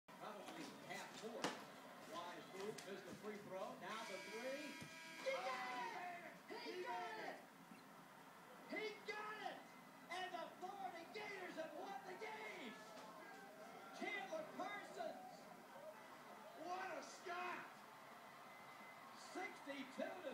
NC State vs Florida shocking OT bomb shot
NCSU lose in overtime thanks to this 70 foot last second shot by Chandler Parsons